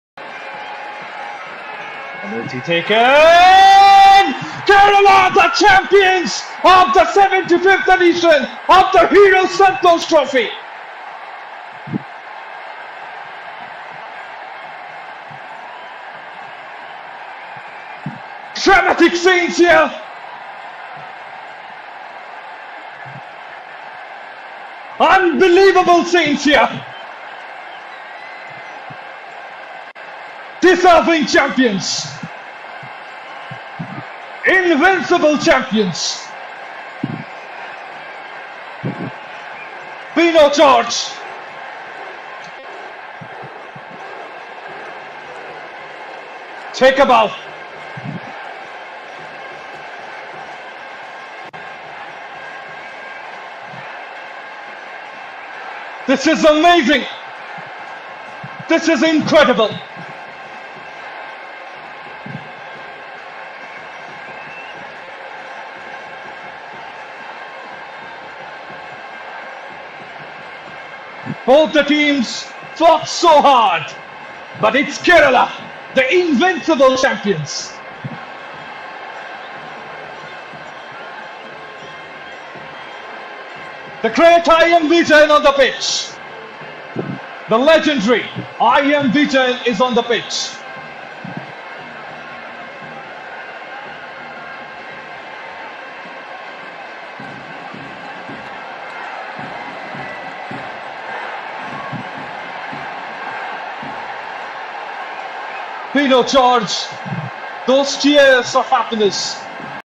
കേരളത്തിന് കപ്പ് നേടി കൊടുത്ത ആ ഗോളും ചരിത്ര നിമിഷങ്ങളും* Santhosh trophy final 2022 -Malappuram
santhosh trophy football # Malappuram *കേരളത്തിന് കപ്പ് നേടി കൊടുത്ത ആ ഗോളും ചരിത്ര നിമിഷങ്ങളും*